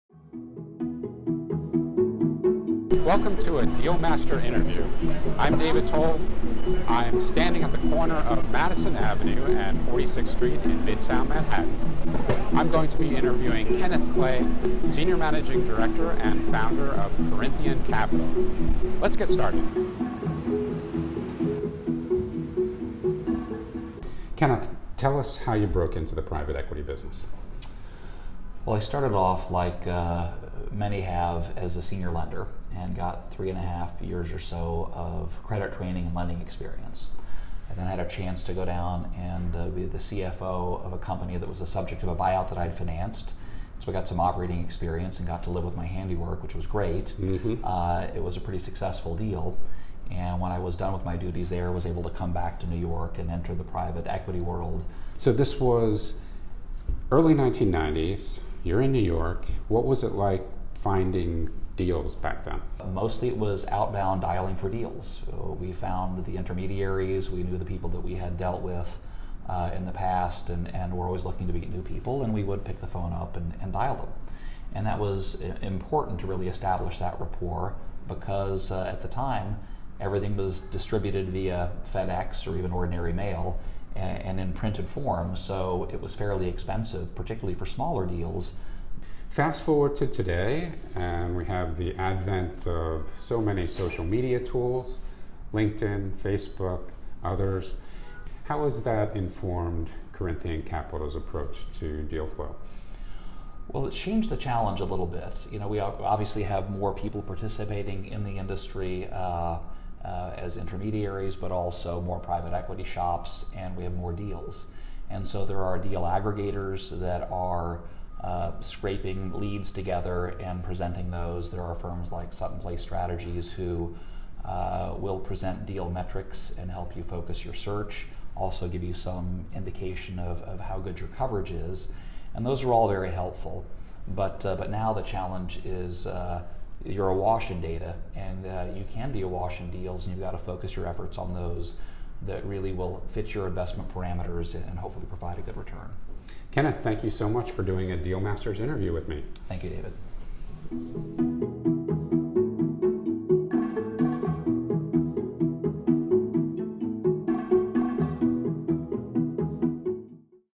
Source Talks ep. 1
Presenting the first video of a new series where we discuss deal origination with PE and M&A pros, in collaboration with David M. Toll.

Interviewed Kenneth Clay of Corinthian Capital Group, who goes into how the current private equity and M&A landscape has shaped deal sourcing efforts.